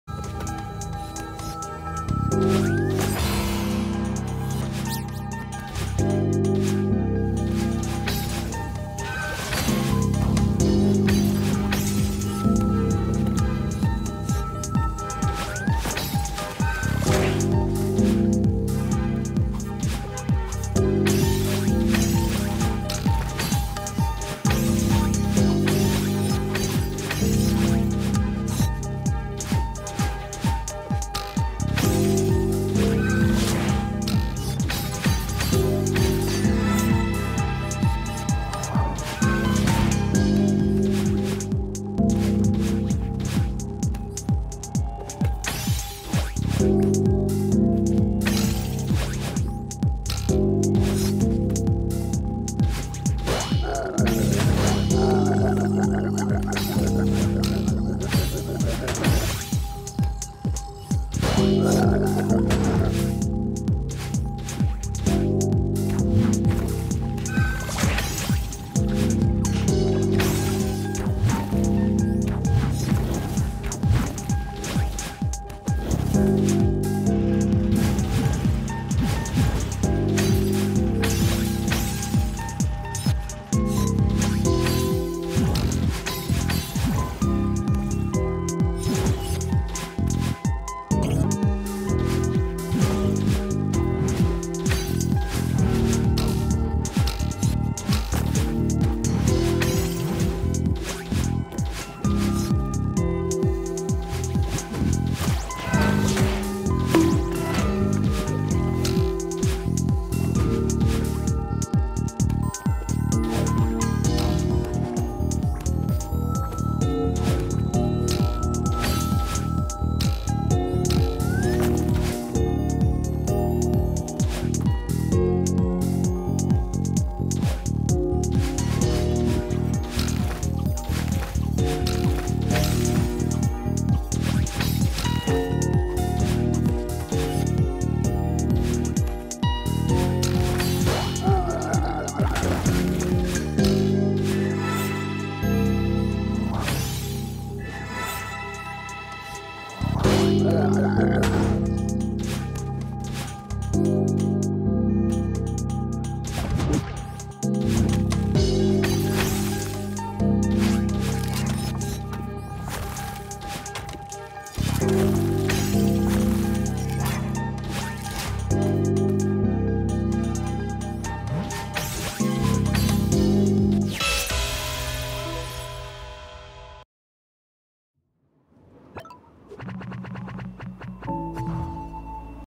Cosmic Reef golden strawberry
This map was made for the Sightreading Conest, and after 9 months of it not releasing/dying Zucchini decided to release it (with a slight bit more polish). This map is a really nice casual experience with some really pretty deco. The golden here is pretty fun aswell I just wish I wouldnt have been straight up braindead in third last room and windsprings room multiple times.